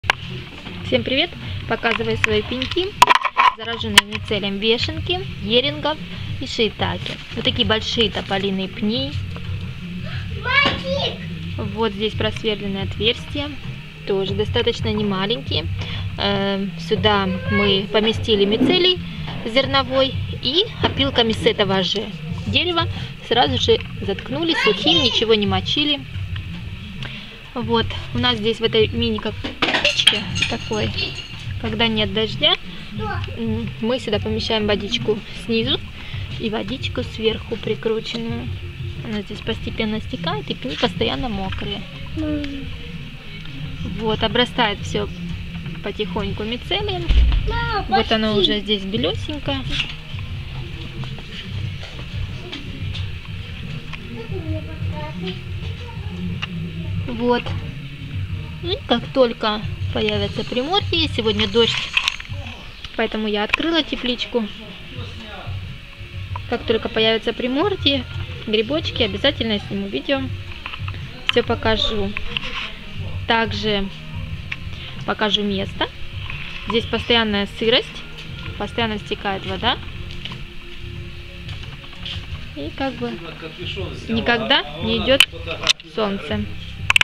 вешенка на пнях
для обильного плодоношения сделана тепличка и обсыпано опилками от слизней